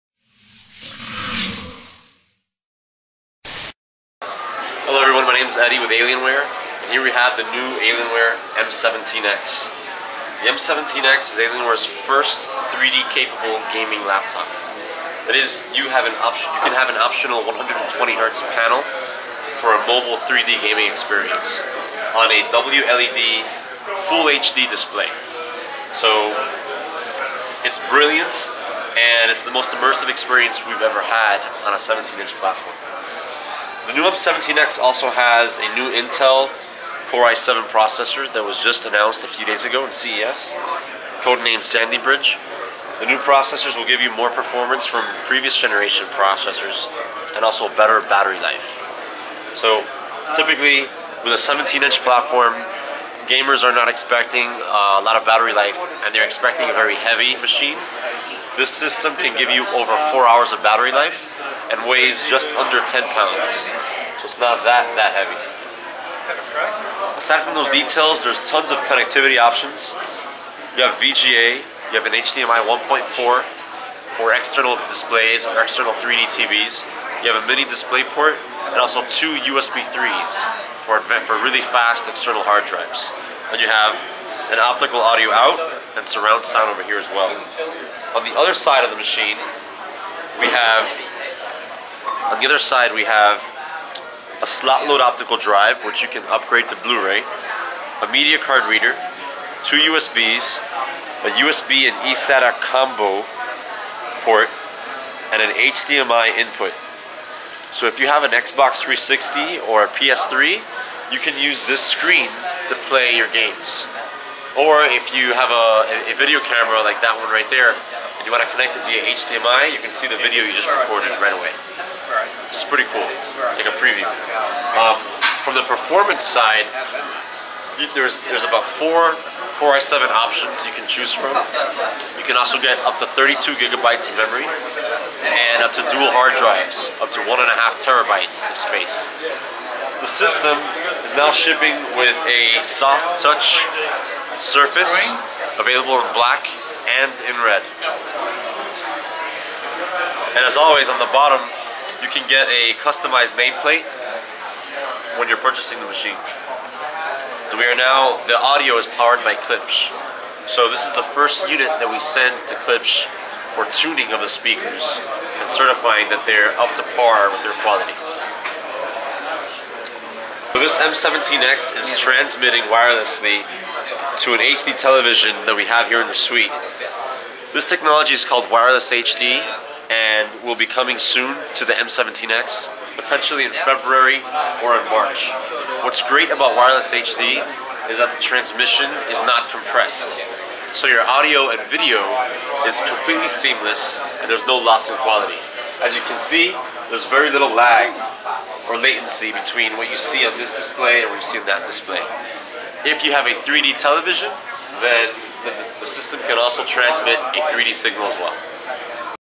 Dell Alienware M17X R3
Preview del notebook da gioco Dell Alienware M17X al CES 2011. Si tratta di una macchina da gioco per enthusiast players equipaggiata con processori Intel Core i7 di generazione Sandy Bridge, doppia scheda video, HDMI in e out e tecnologia Intel Wireless Display. Opzionalmente questo Alienware M17X può essere equipaggiato con uno schermo 3D a 120Hz.